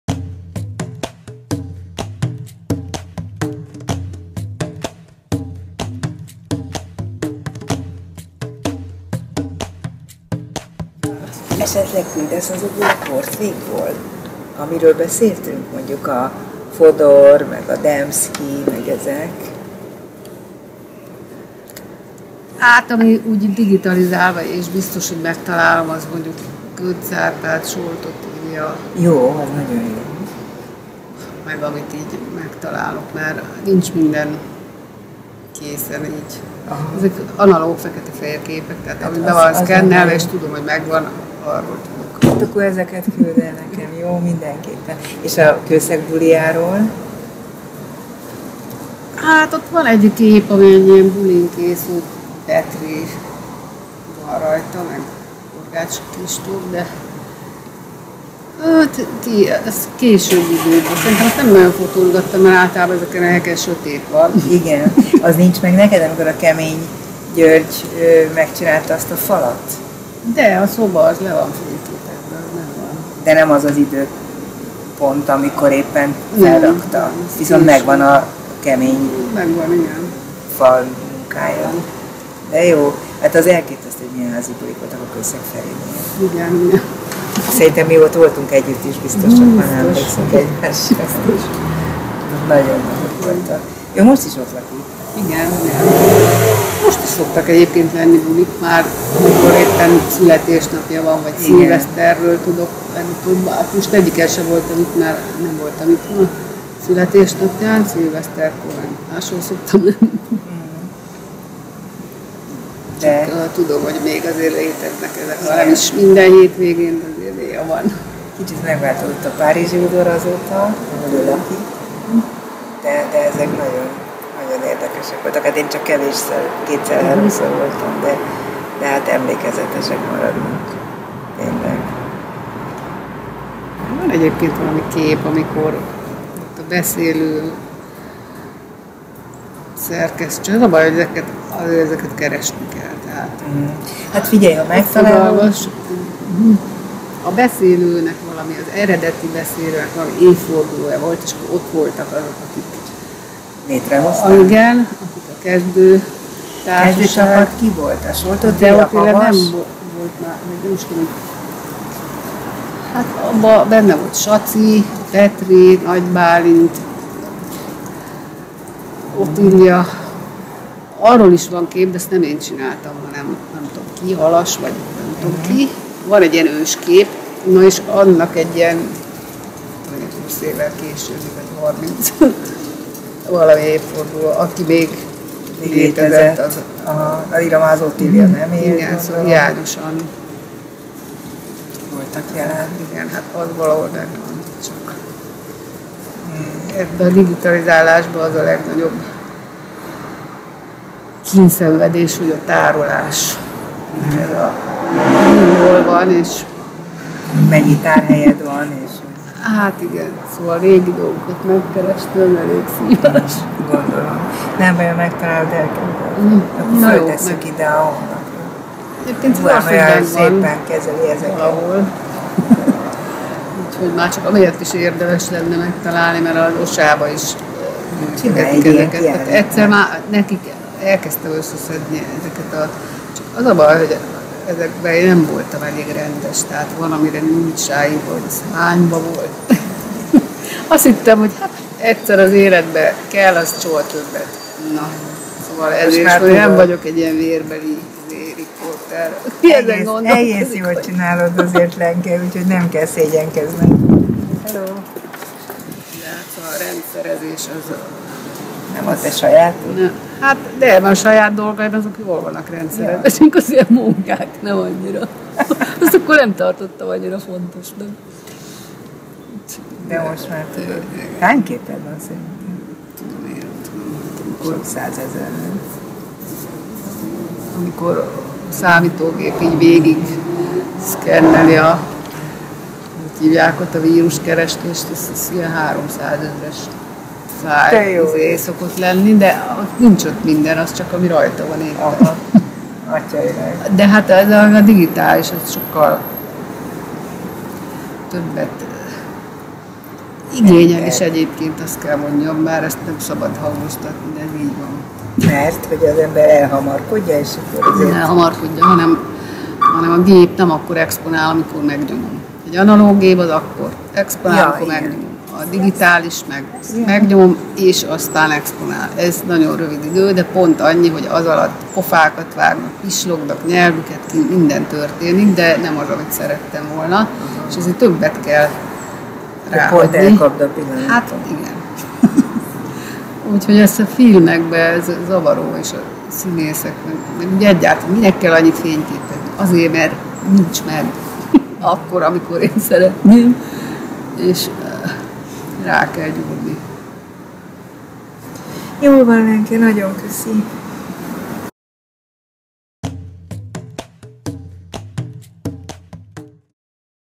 "Egy analóg gép akkor exponál, amikor megnyomom" - Szilágyi Lenke fotóművész (Dobszerda 2020.09.16.)
A kortárs magyar fotográfia emblematikus művésze, stand- és színházi fotós, hegymászó és megszállott utazó. A 80-as évek második felétől folyamatosan fotografálta a hazai underground művészeti élet eseményeit és résztvevőit. A 90-es évektől rendszeresen utazott a (volt) Szovjetunióba, ahol a kommunizmus bukását és a posztszovjet valóságot örökítette meg. Most ezekből a fotókból látható kiállítás a Centrális Galériában, ami szeptember 20-ig még megnézhető.

A Dobszerda vendége Szilágyi Lenke fotóművész, akivel a rádióműsor felvétele után egy rövid kis videót is forgattunk.